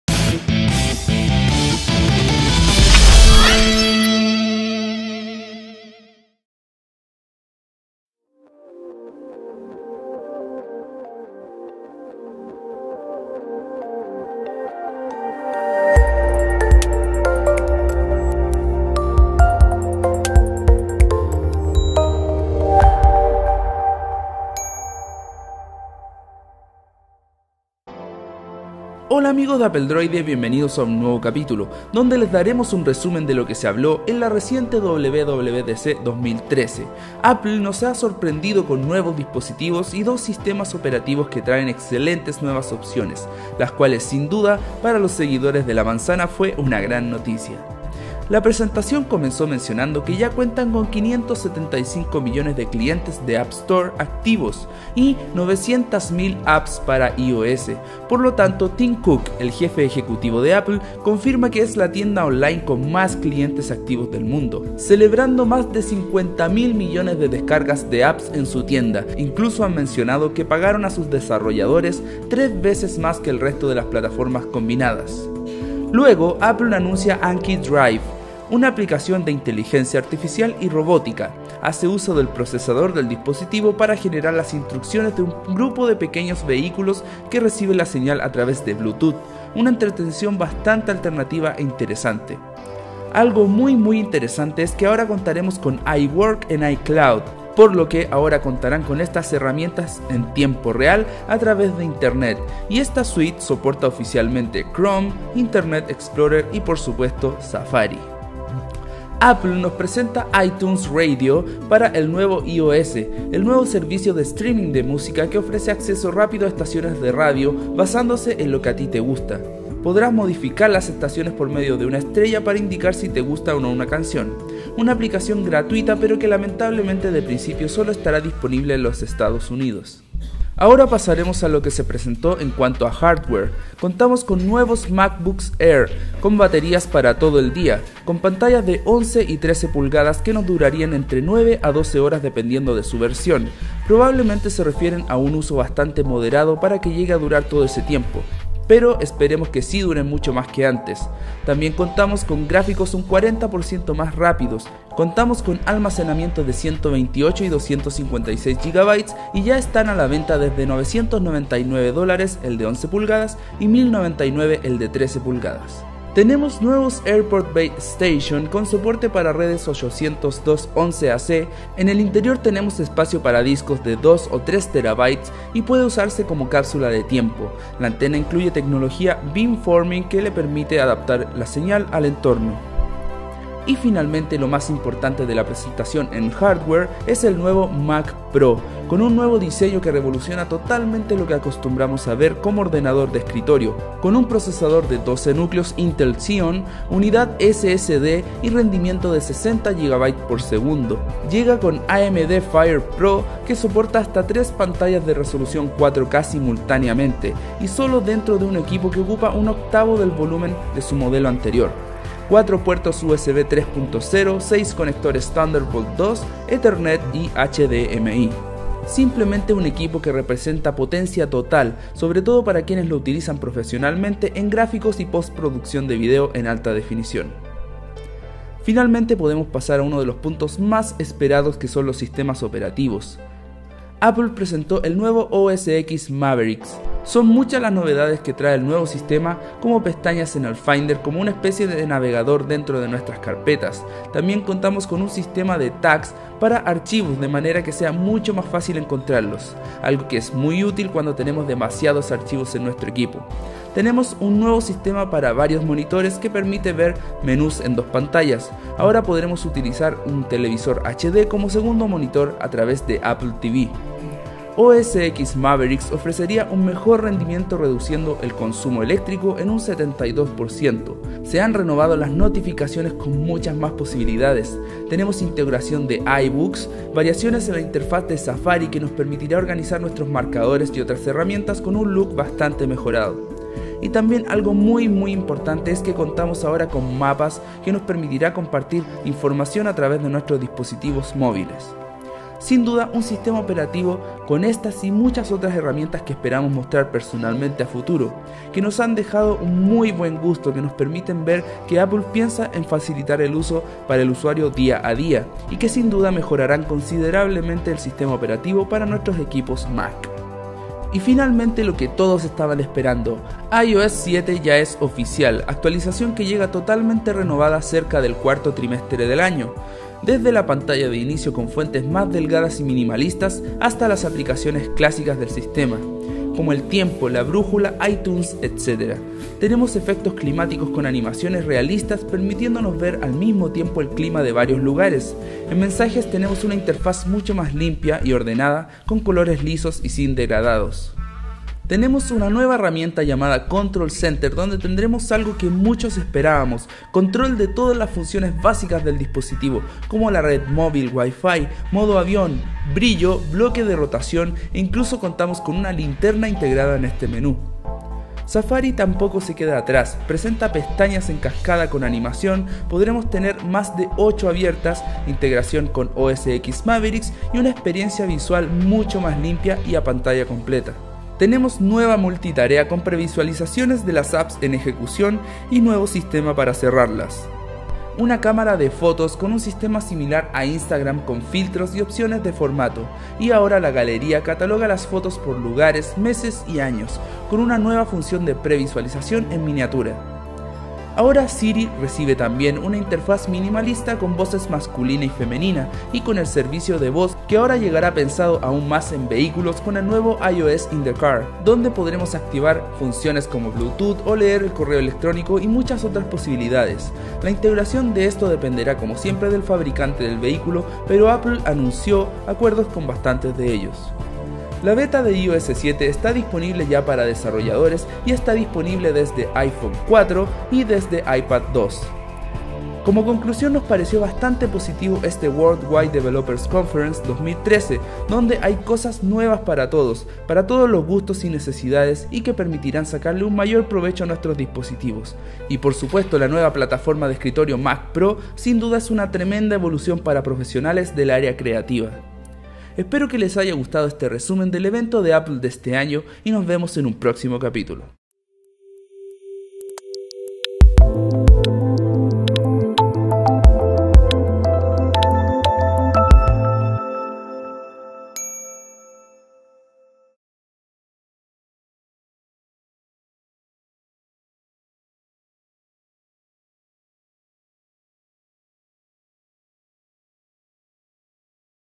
Resumen del evento de Apple WWDC 13 en español
Bienvenidos a un resumen del evento de Apple WWDC 2013 donde podrán conocer detalles sobre los nuevos servicios, hardware y sistemas operativos de este año.